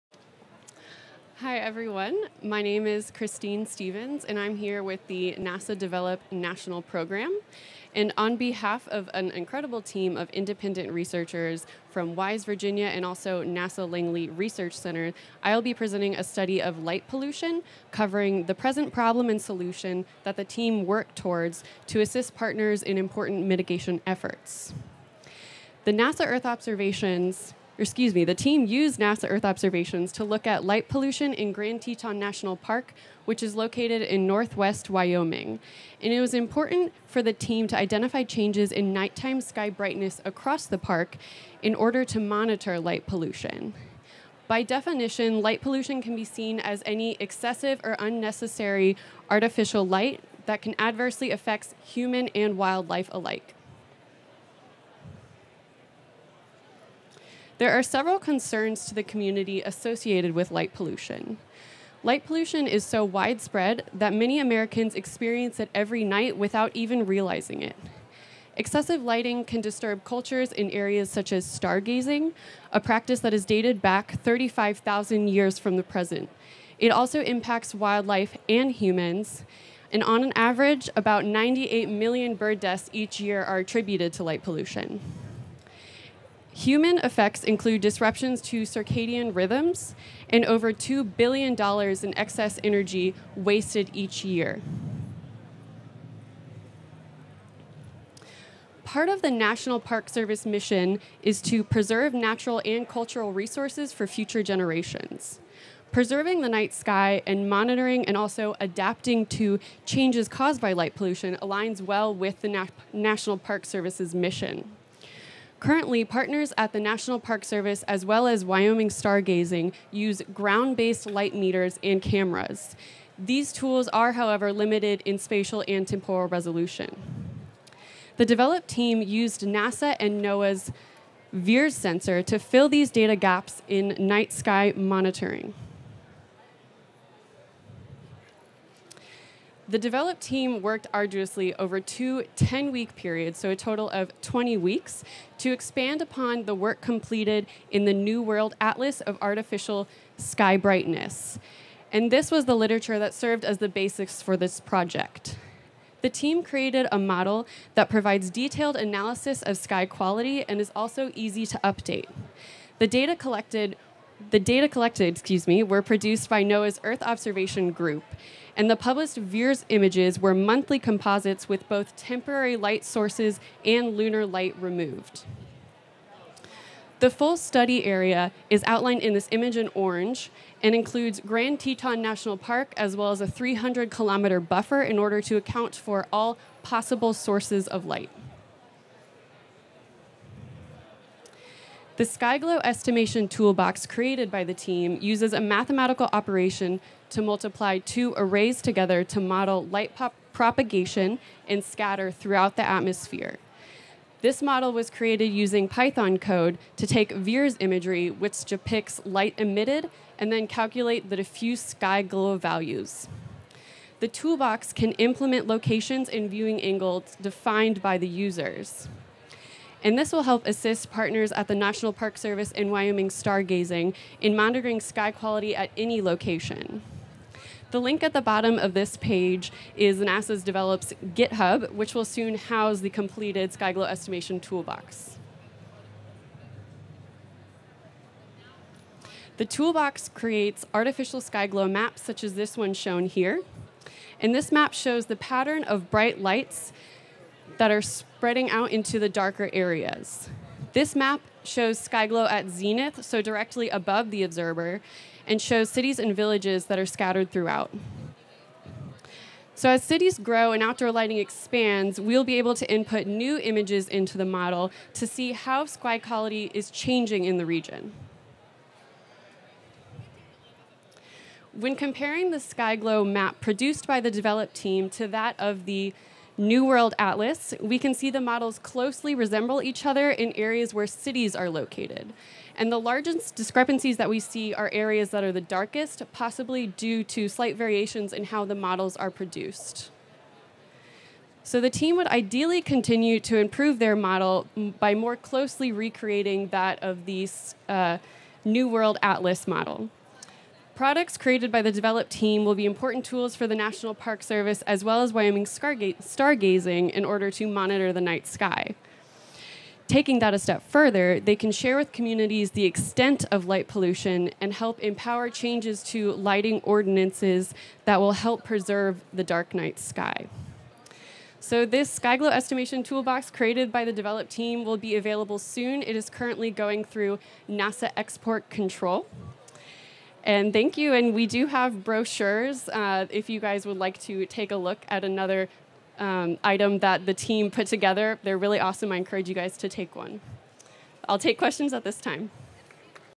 Say No to the Glow by Christie Stevens
Christie Stevens presents Say No to the Glow. Originally presented at the Fall AGU 2017 conference on Thursday, December 14, 2017 at 2:30 p.m.